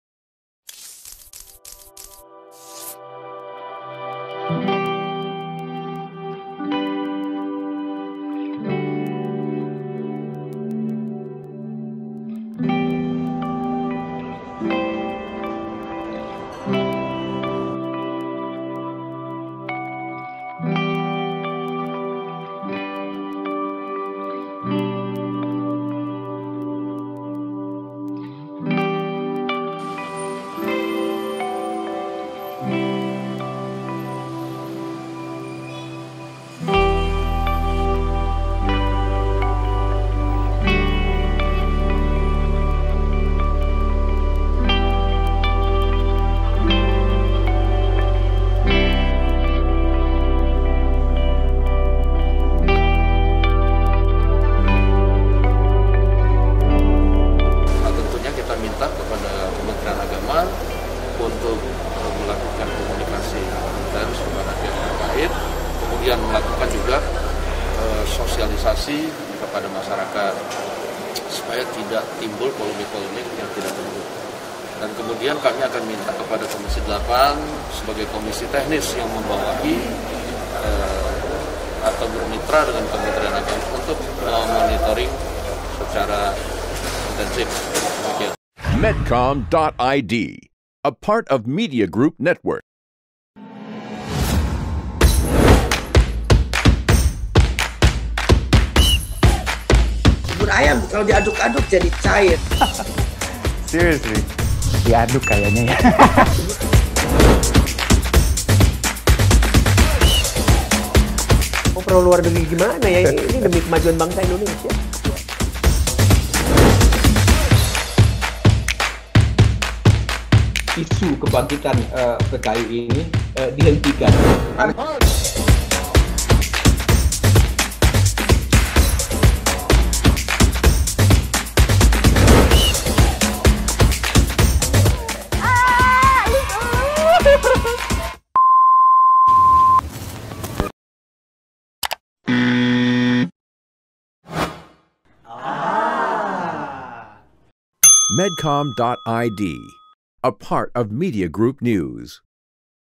Logo Halal Baru Tuai Pro Kontra, DPR RI Angkat Bicara
Wakil Ketua DPR RI, Sufmi Dasco Ahmad meminta Kementrian Agama (Kemenag) mensosialisasikan secara intensif kepada masyarakat terkait logo halal yang baru. Menurutnya, sosialisasi tersebut perlu disampaikan agar tidak menimbulkan polemik di masyarakat.

Maksud dan makna logo baru seharusnya bisa dijelaskan dengan baik guna menghindari salah kaprah dan mencegah pembahasan yang tidak konstrukrif. Sebelumnya, BPJPH Kementrian Agama menetapkan label halal baru yang berlaku secara nasional. Namun, hal tersebut menuai pro dan kontra di publik.